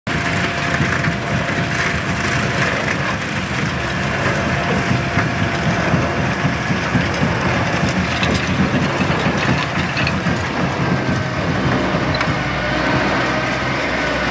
Vervaet, Beet Eater 6 row beet harvester at Kilbrittain Co. Cork. Operator, Niall Crowley